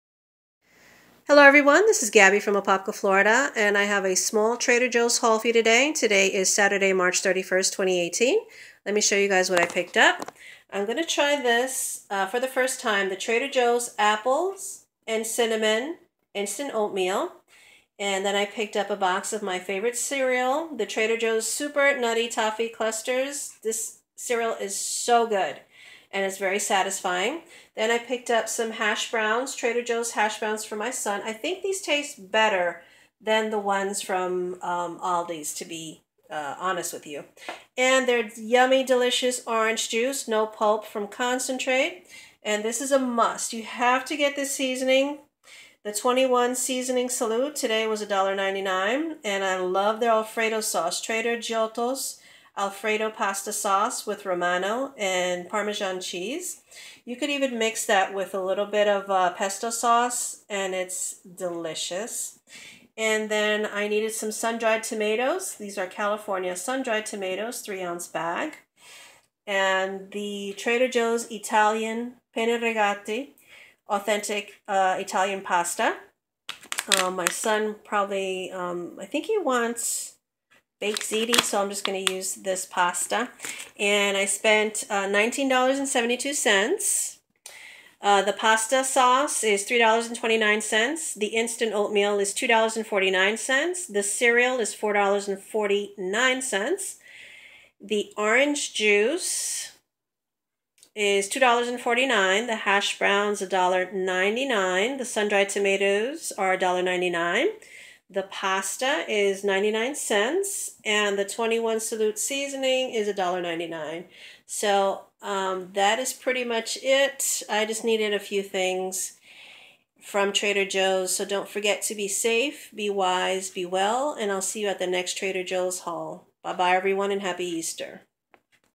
Trader Joe's haul. Saturday, March 31st, 2018.
Happy Easter!🐤🐰
Just a few things from our local Trader Joes. My total for today is $19.72:
Alfredo pasta sauce $3.29
Instant oatmeal $2.49
Cerial $4.49
Orange juice $2.49
Hash browns $1.99
Sun-dried tomatoes $1.99
Pasta pene regate $.99
21 seasoning salute $1.99
I hope you have a wonderful weekend and a joyous Easter...😉🐰🐤